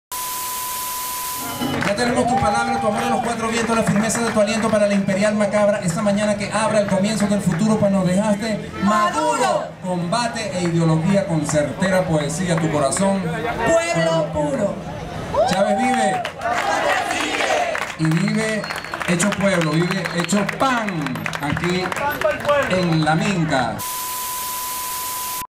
Catia Tve - Promo Colectivo Un Canto
Cantores toman @laminkaeps en apoyo a las nuevas formas de producción. Contra la geurra económica @tostaitoLeo @uncantoria